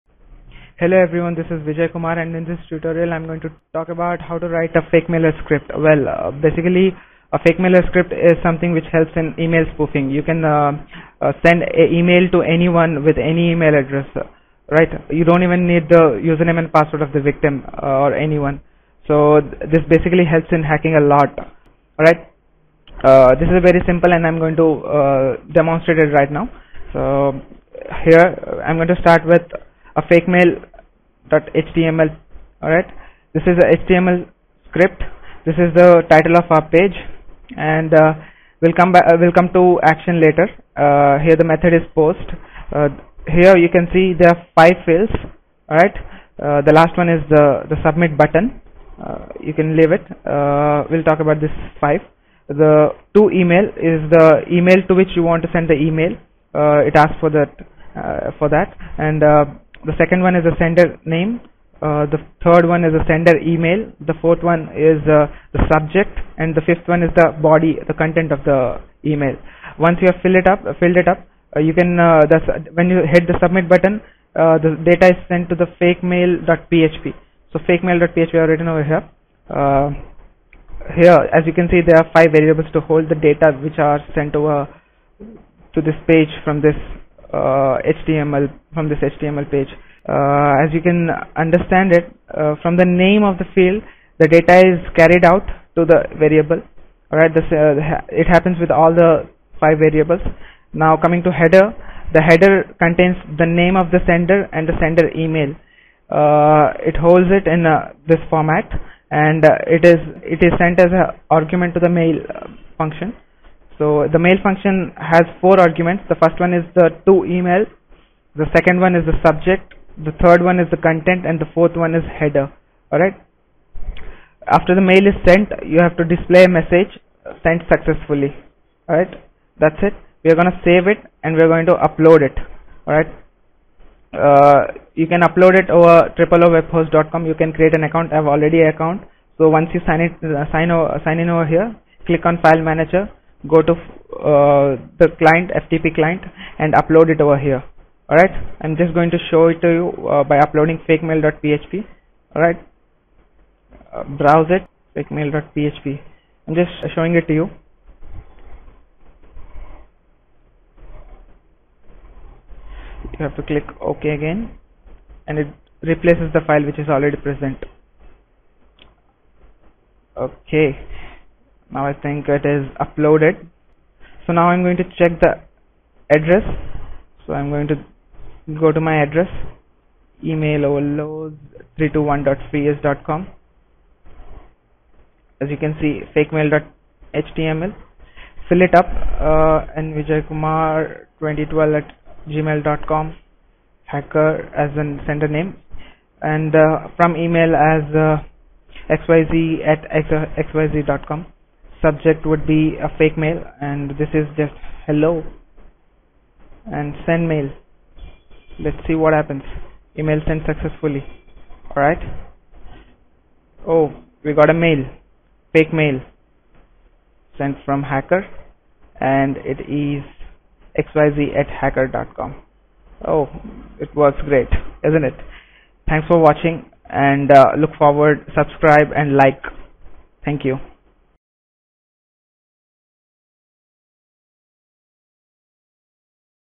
Email Spoofing with Fake Mailer script
This video demonstrates how to write a fake mailer/ email spoofing script in PHP. This tutorial is for beginners but expects a little prerequisite knowledge over HTML.

Check the script in the comment section.